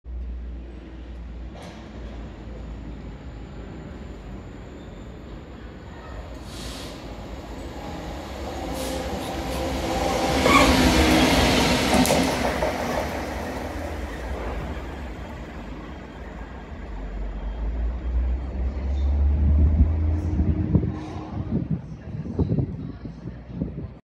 Colas Rail Class 66 no: 66846 @ Newport (0Z51) 11/11/2024.
0Z51 1207 Westbury Down T.C. to Baglan Bay (Colas).
Light Locomotive.
Pathed as Diesel locomotive.